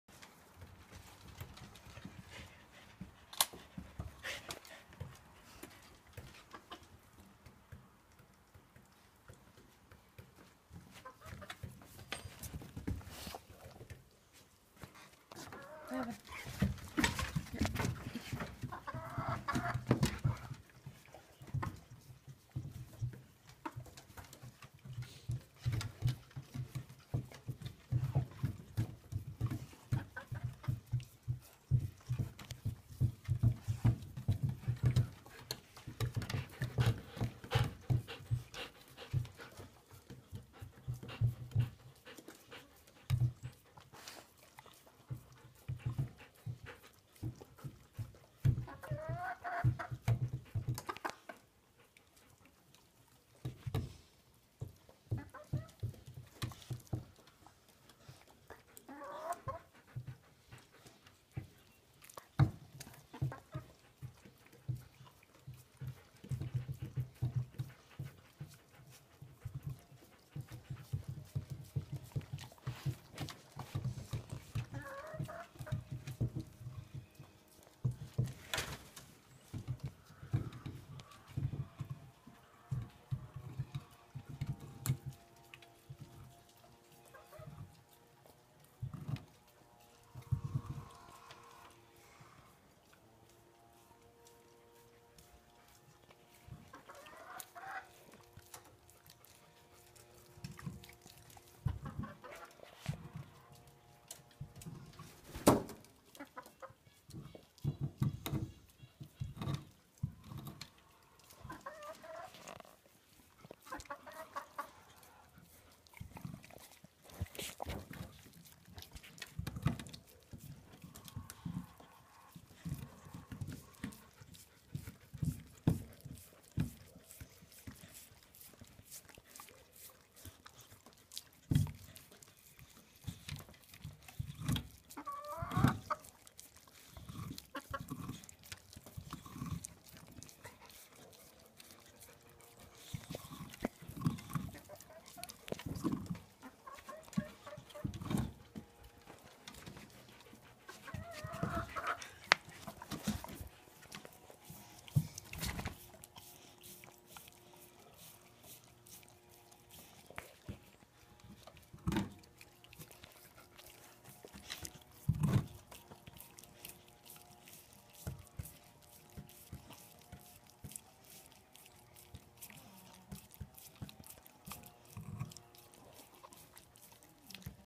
Frozen treats
Our friendliest chicken is getting in on the action too. Balloons didn't work work me so I filled metal bowls with water, pieces of dog biscuit and frozen mixed veggies.